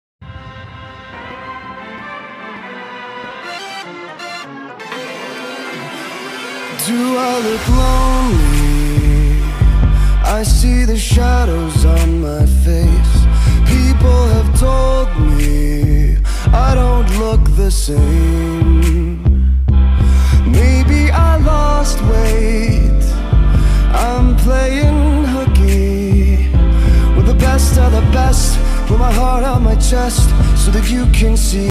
Give Meh Dares read description
UwU sorry for not posting lately I have some pranks and gacha stuff I plan on doing the girl that's talking is me irl ya I died my hair and i am coming up with a GCMV for part 3 of my gaming life GCMV